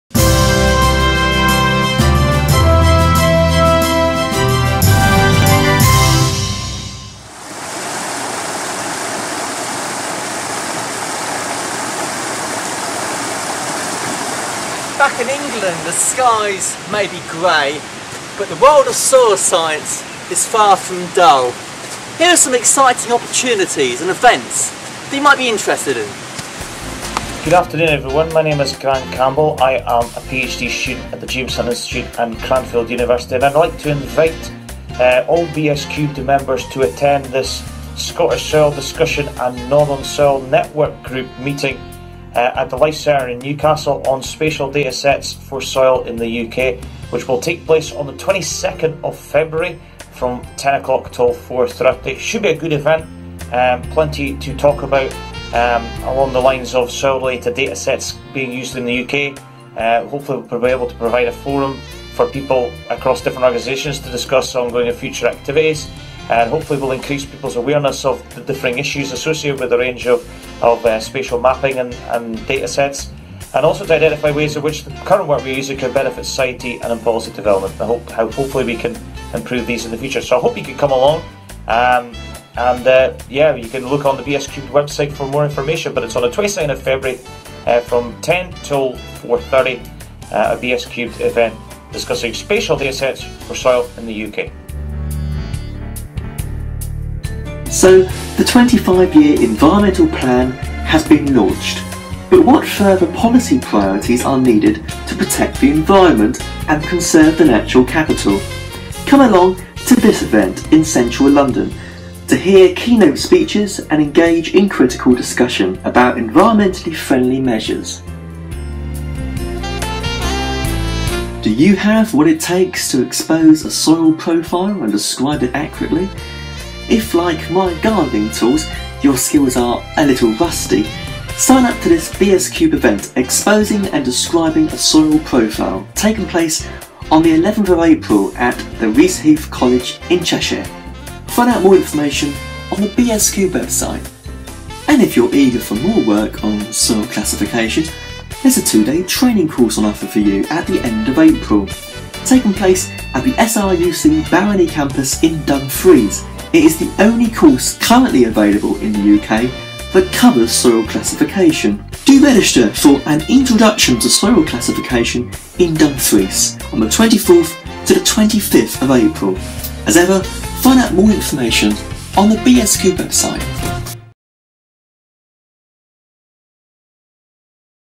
MiniPEDcast Jan 18- Soil Science around the UK
A bite-sized chunk of the PEDcast in January 2018. In this segment, Grant Campbell shares news of an exciting event taking place in February and Dan presents a selection of other pedological events coming up soon.